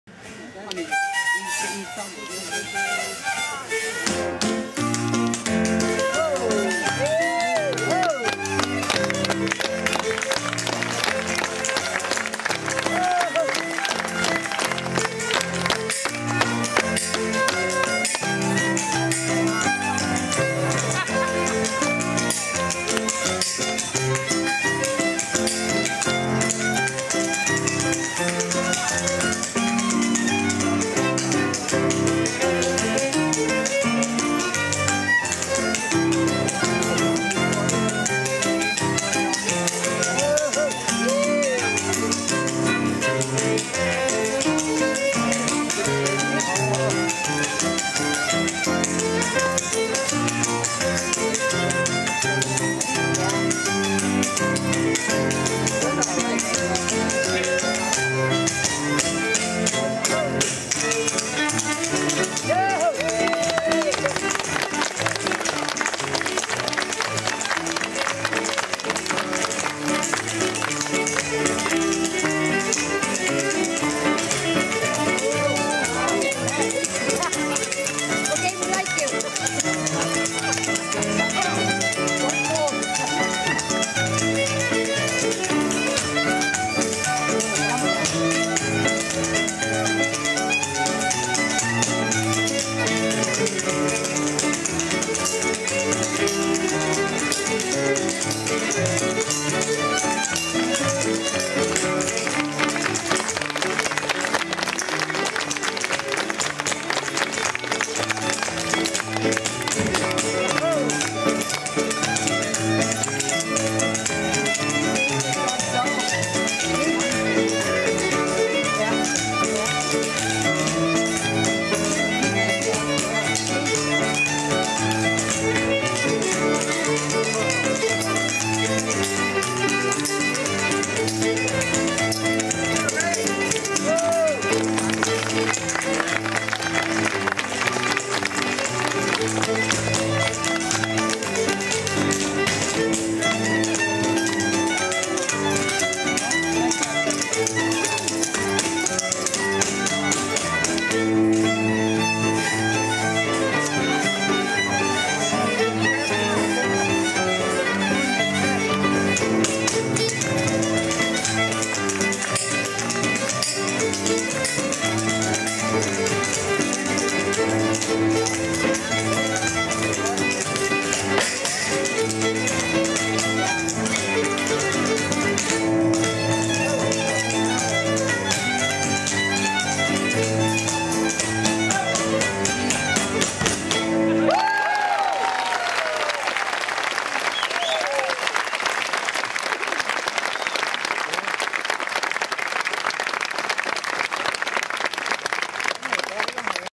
Copy of Shanty Music Show Pembroke Ontario, Broad Axe Dance August 4 08
Peter Dawson on fiddle accompanied by Carol Kennedy on Piano .
Stepdancing by Barry Gilchrist